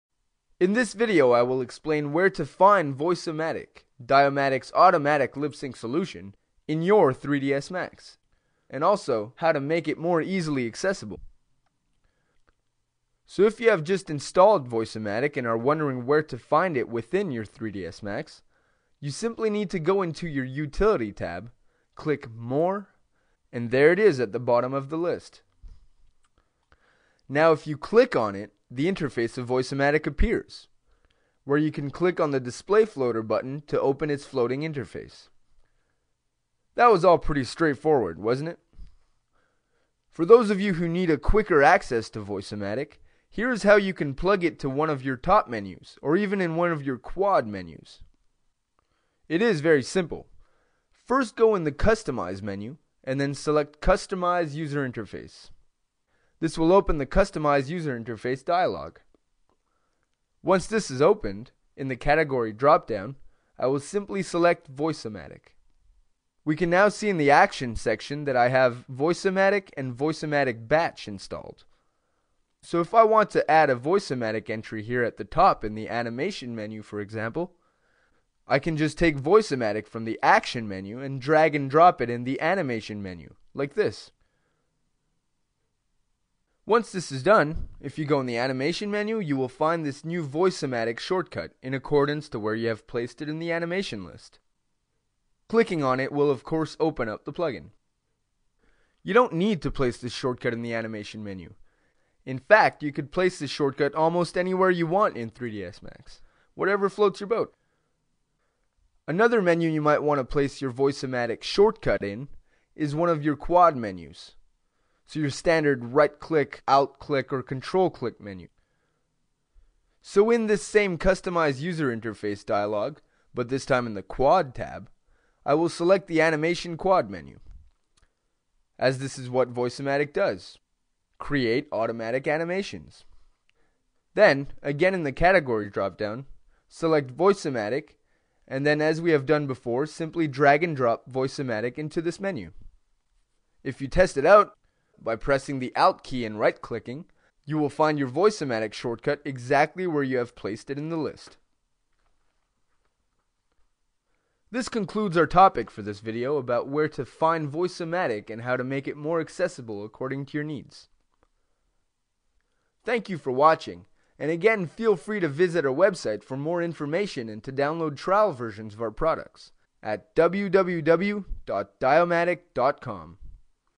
Voice-O-Matic: where is it in Autodesk 3ds max?
This video depicts where to find Voice-O-Matic within your 3ds max and also how to make it more accessible for you to use.

Get more information about Di-O-Matic automatic lipsync solution and download the trial version